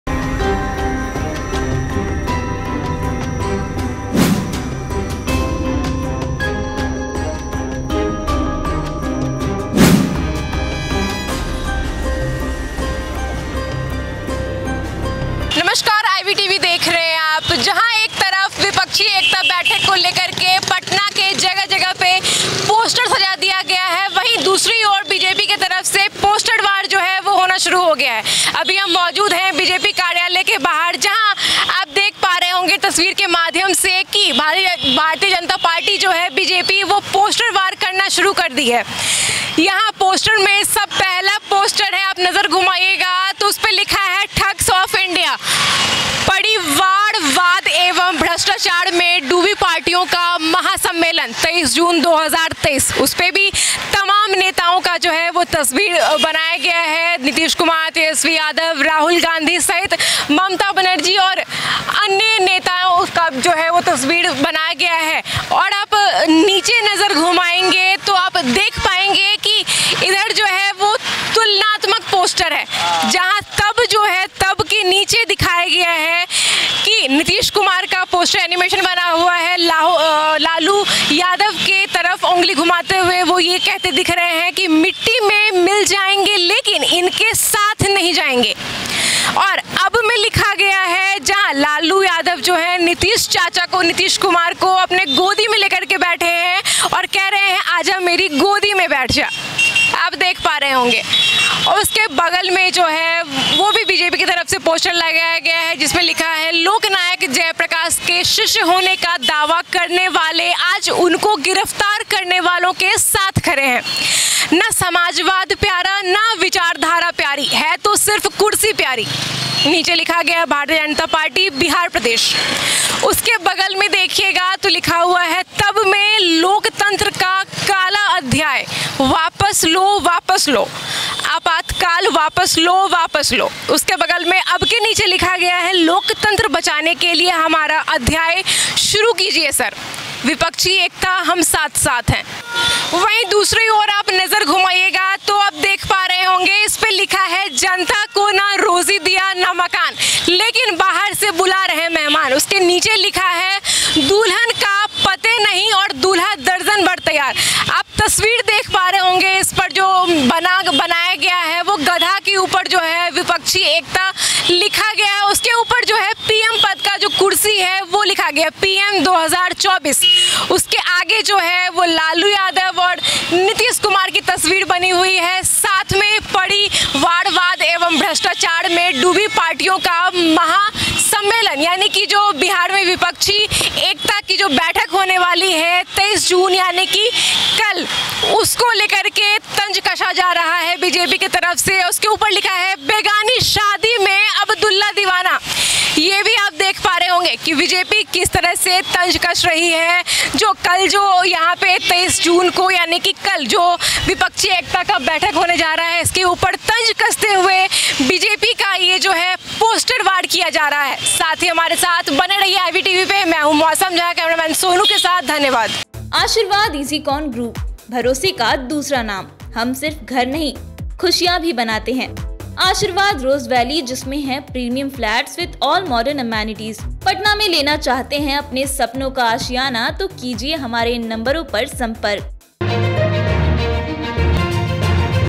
अरे ये क्या! BJP दफ्तर में लग गया Nitish, Laluऔर Tejaswi सहित विपक्षी नेताओं का poster,देखिए....
अरे ये क्या! BJP दफ्तर में लग गया Nitish, Laluऔर Tejaswi सहित विपक्षी नेताओं का poster, देखिए, क्या क्या लिखा है